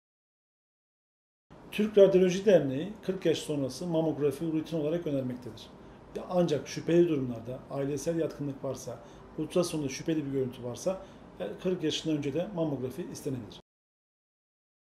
Meme kanseri tanısı için mamografi kaç yaşından itibaren yapılmalıdır? - Doç. Dr. Bülent Çitgez
Türk Radyoloji Derneği 40 yaş üzerindeki kadınların, her yıl mamografi çektirmesini önermektedir. Mamografi 40-70 yaş arasında yılda bir tavsiye edilmektedir.

Doç. Dr. Bülent Çitgez
Genel Cerrahi Uzmanı